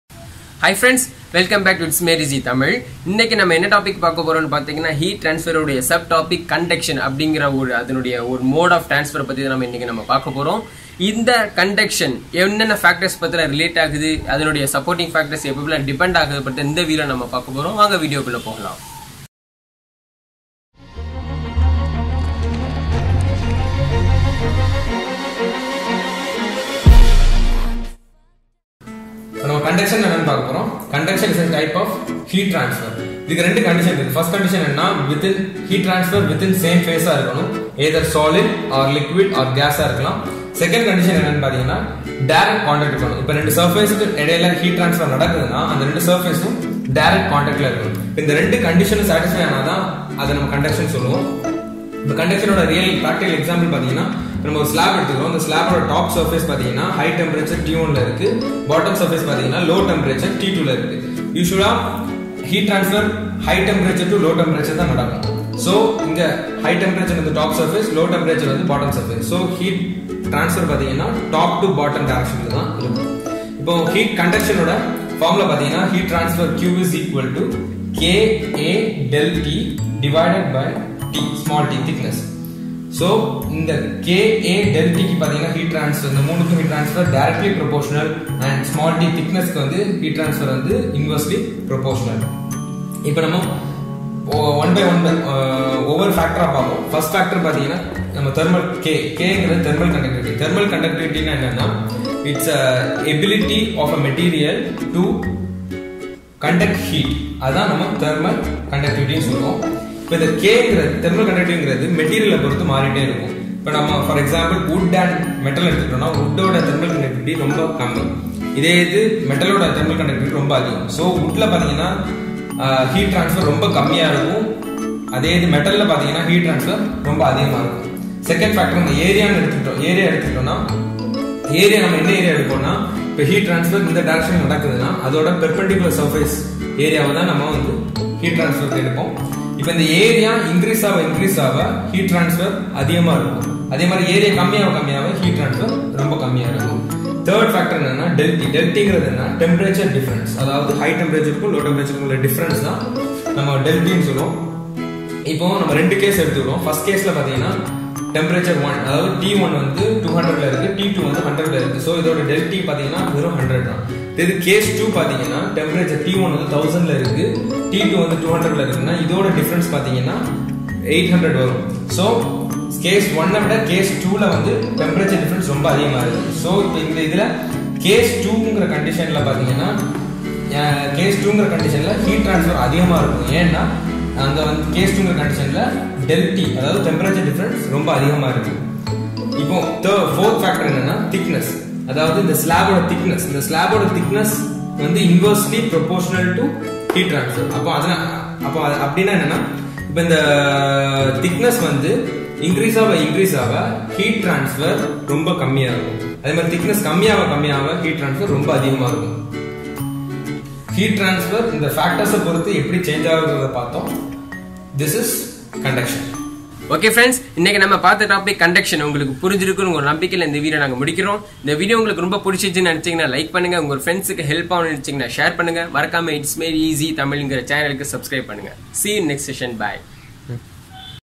What is CONDUCTION? #09 | TAMIL | It's_made_EZy
Simple and brief explanation on CONDUCTION in HEAT TRANSFER by Its_made_EZy.